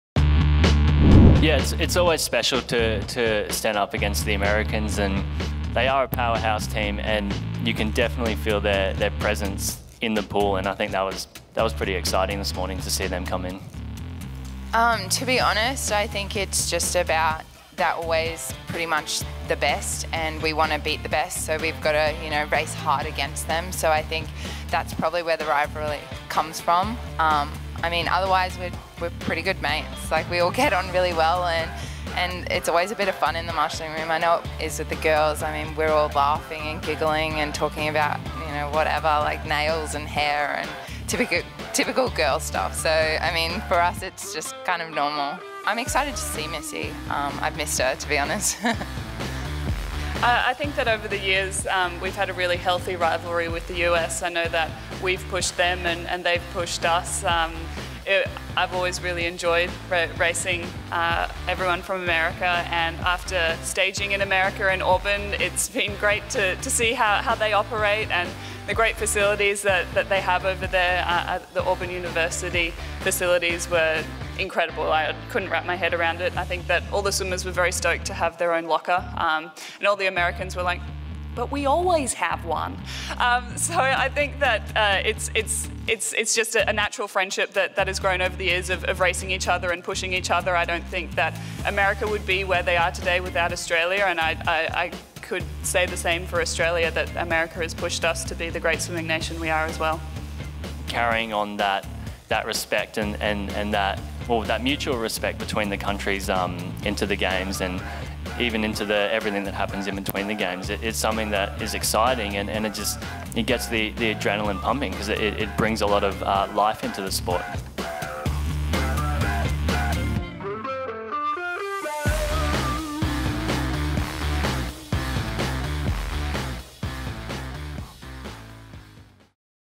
The age-old rivalry in the pool: USA vs. AUS
Australian swimmers Cameron McEvoy, Emily Seebohm and Cate Campbell speak about the rivalry and friendship between the United States' and Australian swim teams.